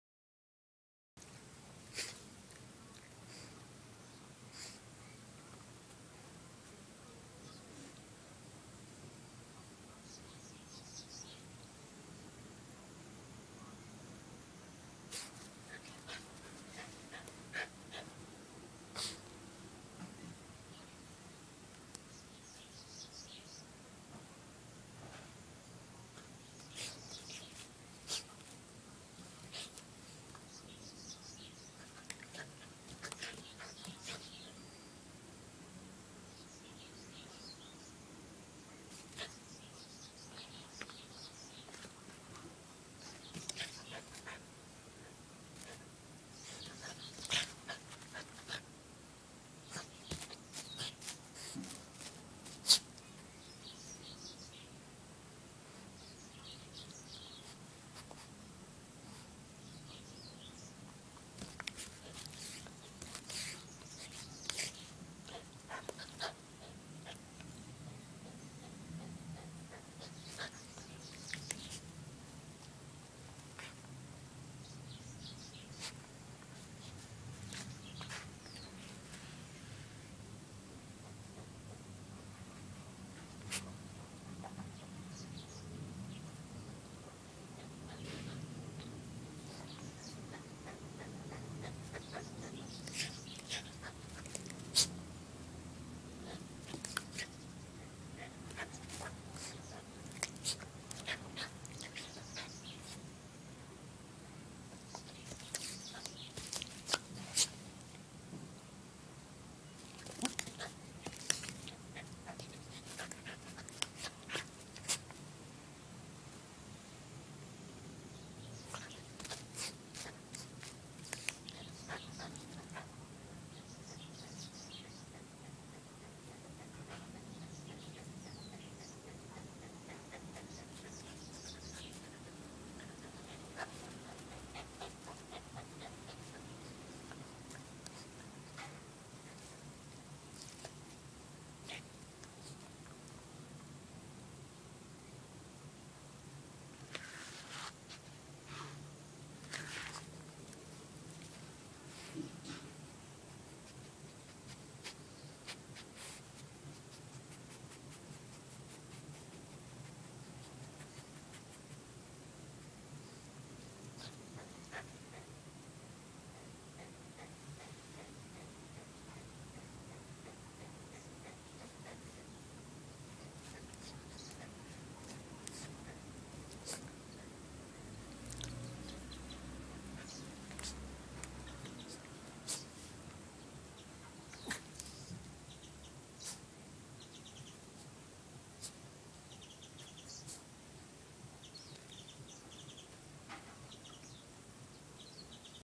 トイプードルと虫 (Toy Poodle fighting with insect)
恐る恐る暴れまわっています。
Timidly fighting with insect.